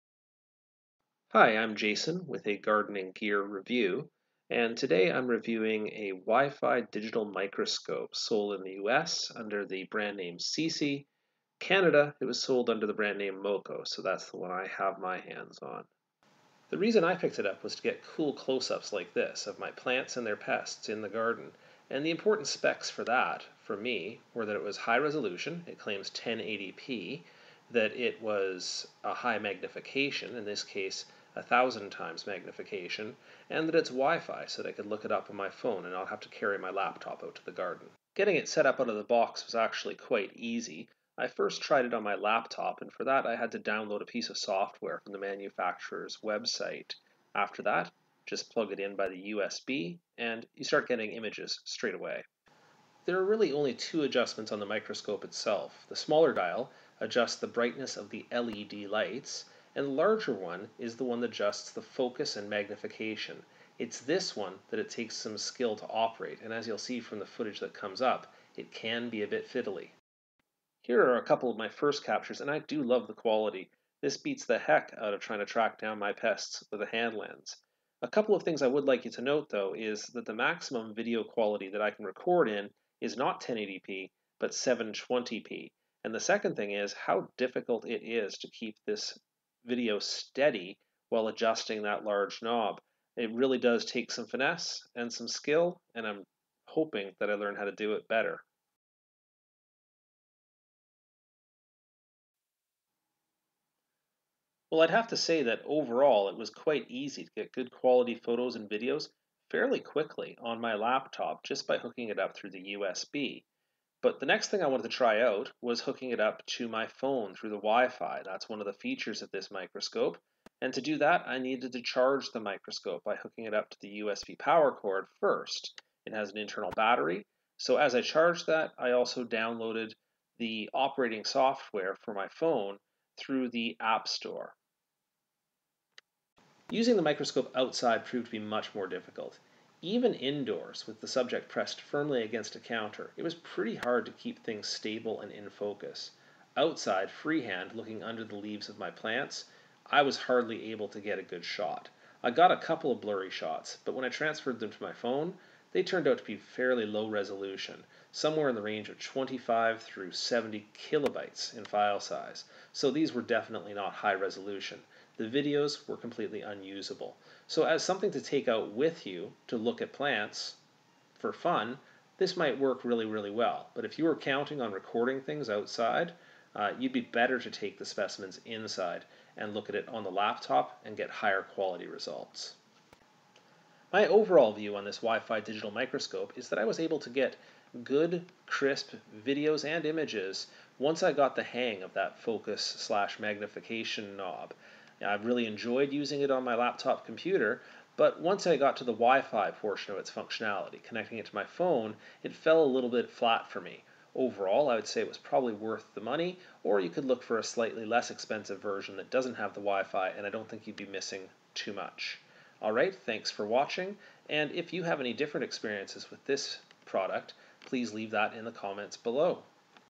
Seesi WiFi USB Digital Microscope Review
I selected this Digital Microscope for scouting insect pests in my garden and greenhouse. The brand in the US (Amazon) is Seesi, here in Canada it's marketed as MoKo. The microscope is made in China, and the supplier website is a company name Inskam. Here are the claimed specs:

1080p resolution, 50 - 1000x magnification, 2MP, 8x LED lights, built-in 900mAh battery, compatible with iOS, Android, Windows, Mac.